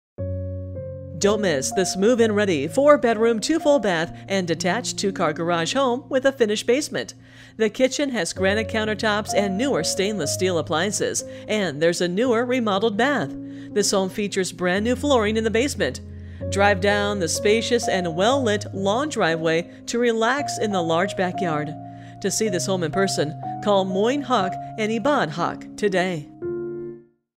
539 S Wisconsin Avenue Villa Park, IL | ColdwellBankerHomes.com
539 S Wisconsin Avenue, Villa Park, IL

LOCATION! LOCATION! AND LOCATION!

FOUR BEDROOM, TWO FULL BATH AND A TWO DETACHED CAR GARAGE WITH FINISHED BASEMENT. READY TO MOVE IN. GRANITE KITCHEN COUNTERTOP, NEWER STAINLESS STEEL APPLIANCES, NEWER REMODELED BATH. FRESH INTERIOR AND EXTERIOR PAINT, LARGE GOURMET KITCHEN WITH CERAMIC FLOORS! BRA